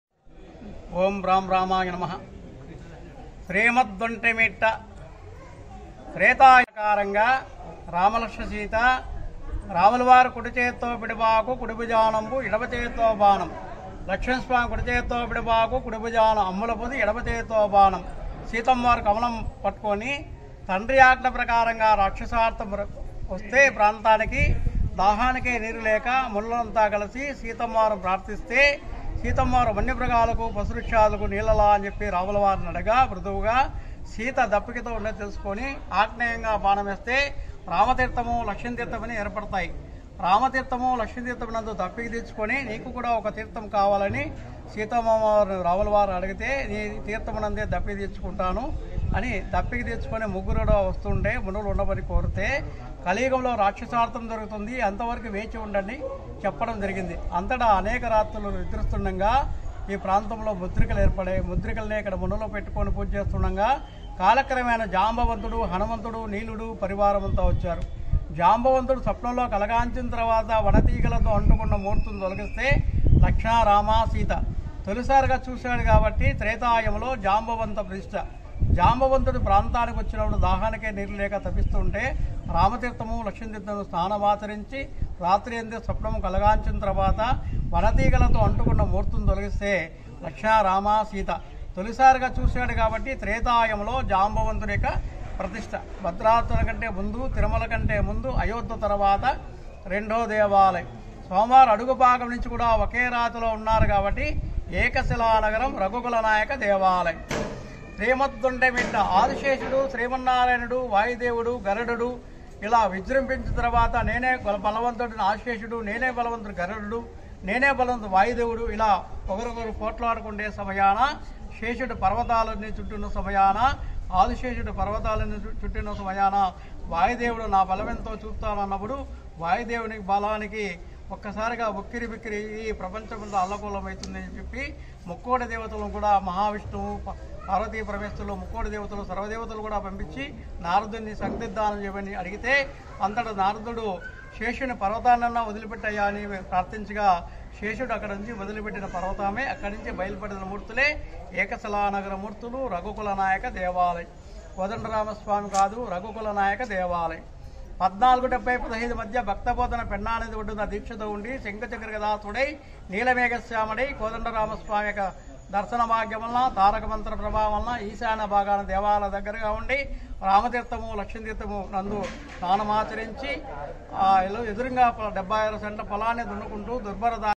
Some Lesser Known Facts About The Iconic ‘Vontimitta Kodandarama’ Temple In Kadapa, AP || RambantuTv
Some Lesser Known Facts About The Iconic ‘Vontimitta Kodandarama’ Temple In Kadapa, AP